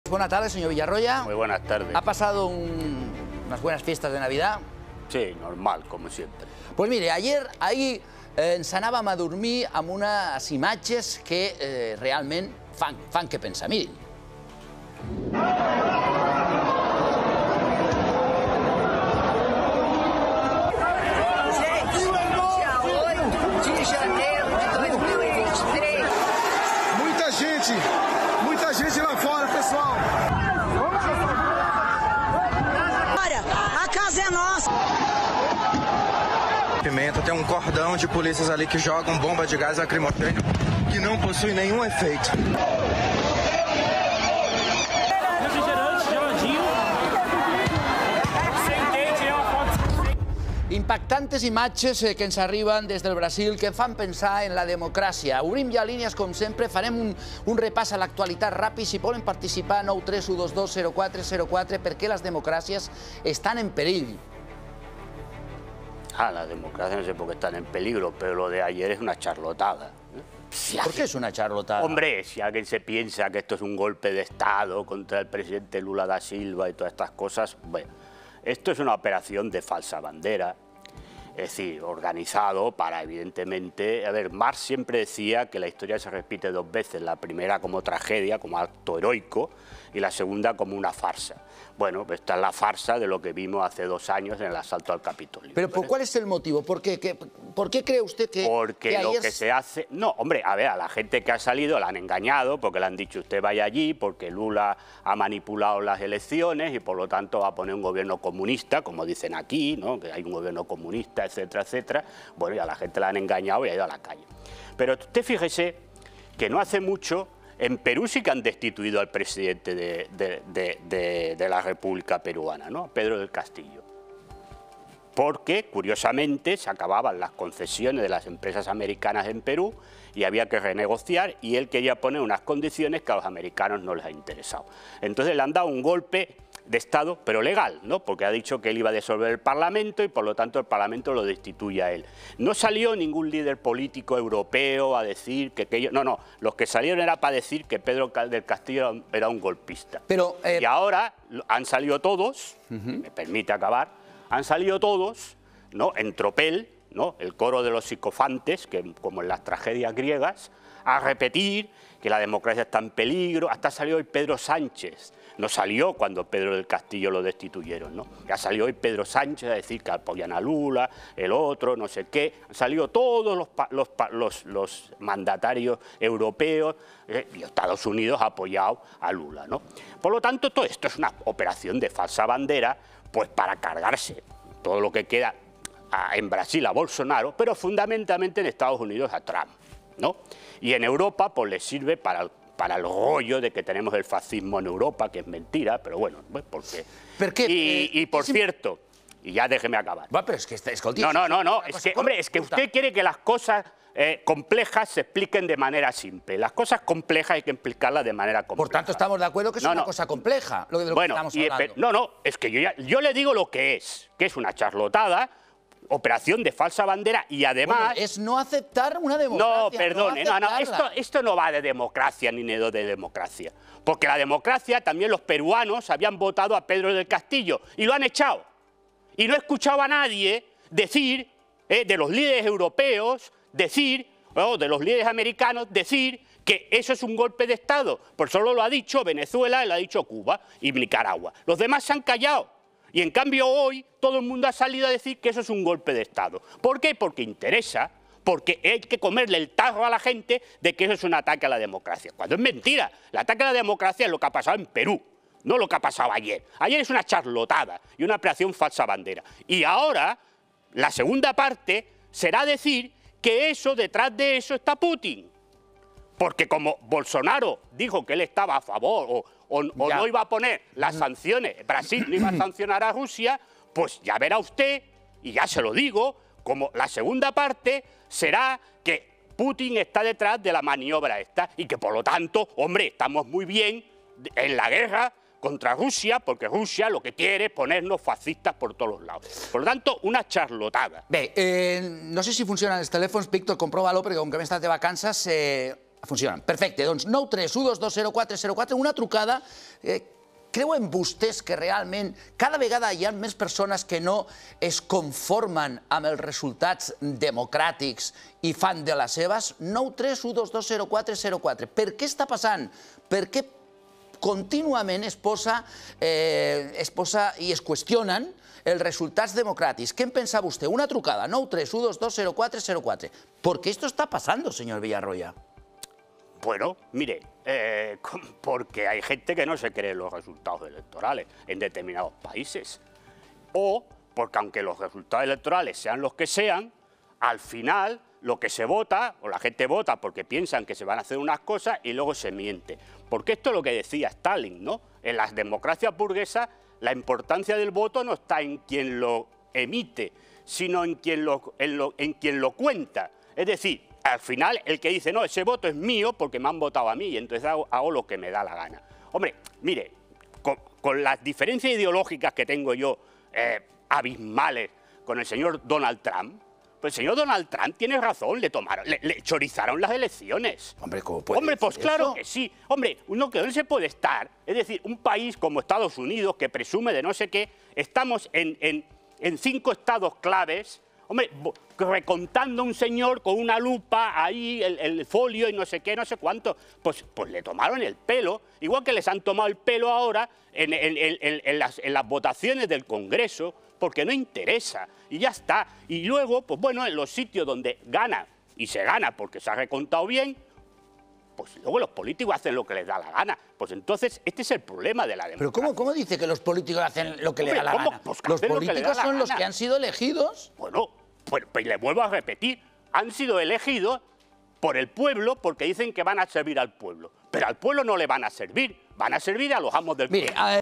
Villarroya sobre el problema a Brasil i Perú | OpinaCat
Per què les democràcies estan en perill?

Debat amb José Miguel Villarroya a l'OpinaCat de Carlos fuentes